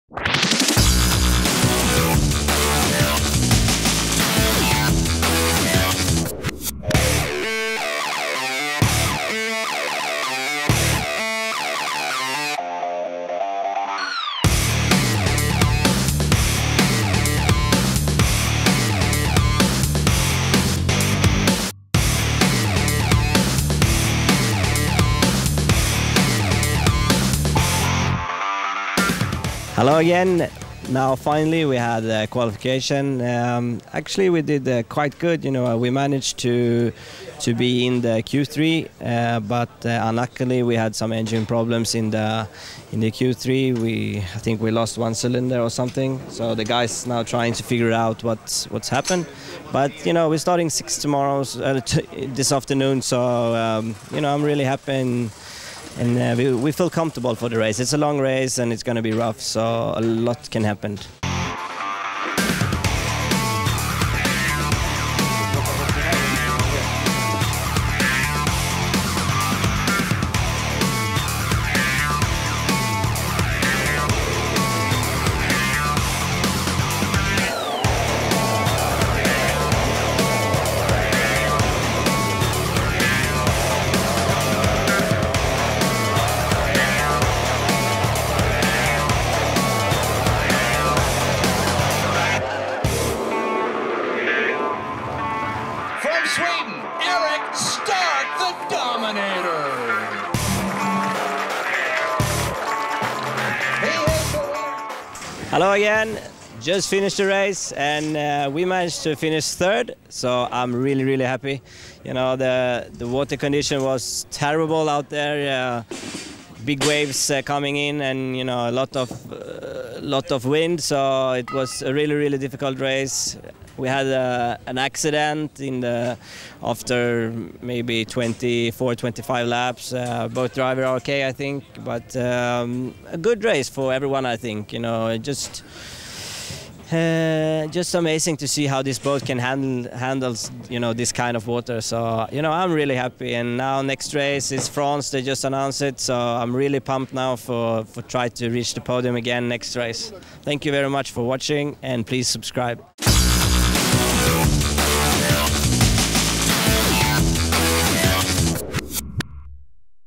Erik Stark Vlog F1H2O GP Qatar 2015 Showdown
Erik Starks Vlog from the F1H2O Grand Prix of Qatar in Doha 2015